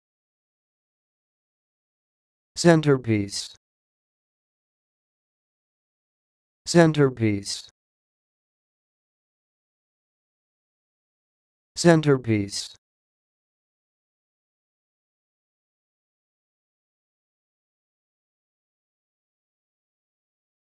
Learn how to say this word: "Centrepiece"
This is the definition of "Centrepiece":
n.  An ornament to be placed in the center, as of a table,
   ceiling, atc.; a central article or figure.